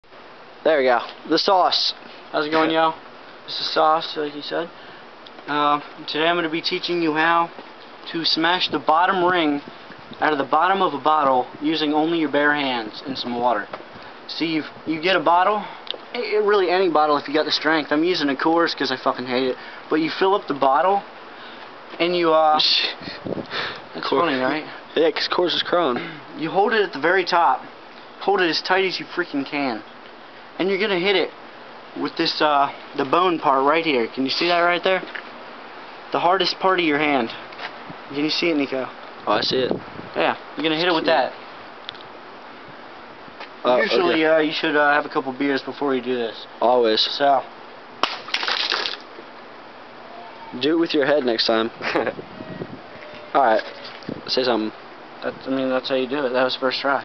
Sauce Beer Reviews: How To Break A Bottle With Your Bare Hands
nice party trick to learn. the trick is hit it without holding back. hit it in the right spot or it will hurt. if you do it right and it breaks it doesnt hurt at all. grip tight and wack the fucker